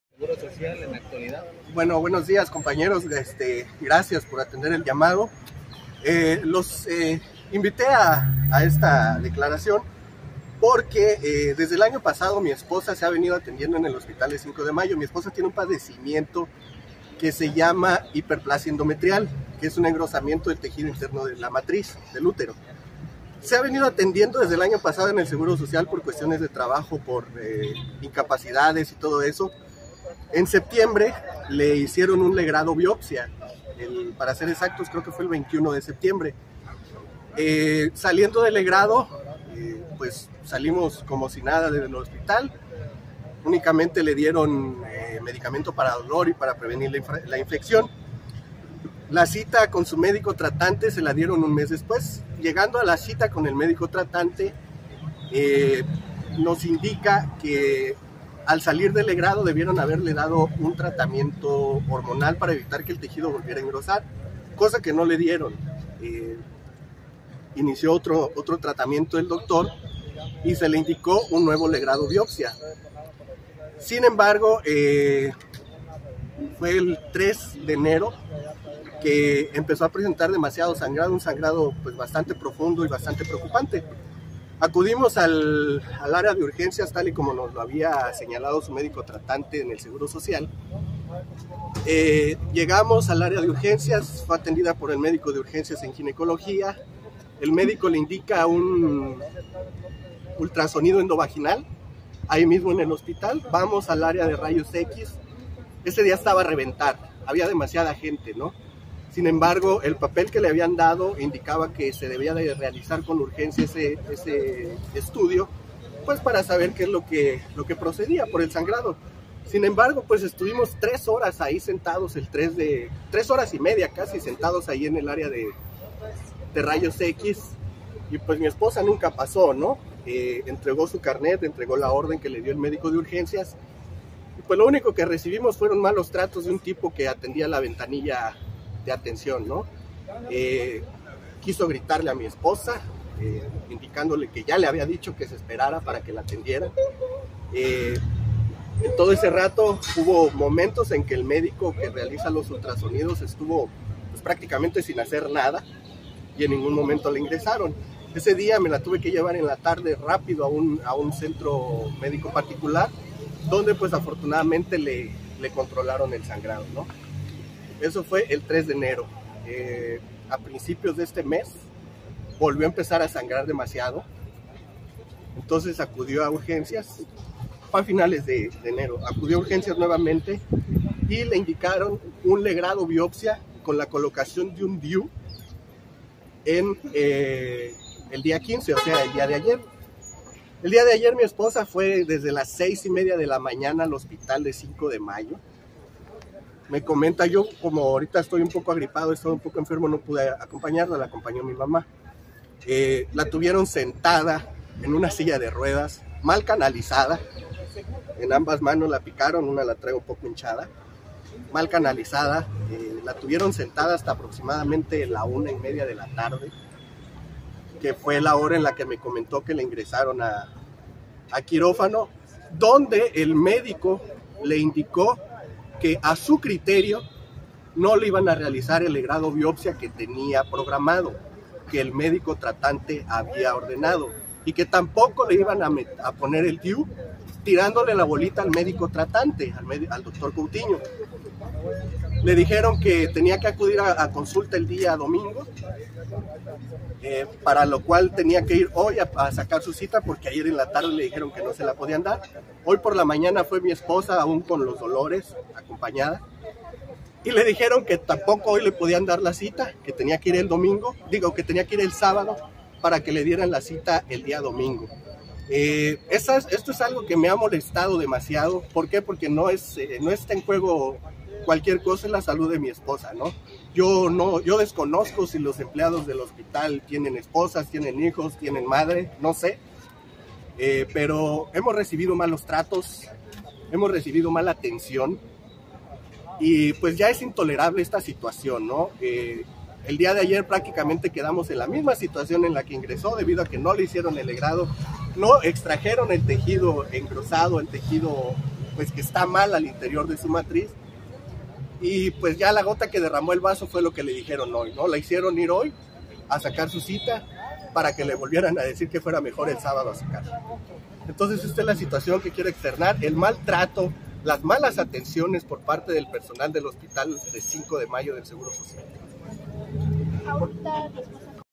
Alex Alcázar denuncia que su esposa no ha tenido atención adecuada en hospital del IMSS
Jesús Gómez/ASICH

Alex Alcazar denuncia  que su esposa no ha tenido una atención adecuada en el hospital general de zona 2 del IMSS, conocido como 5 de Mayo en Tuxtla Gutiérrez, pese a que ha presentado sangrados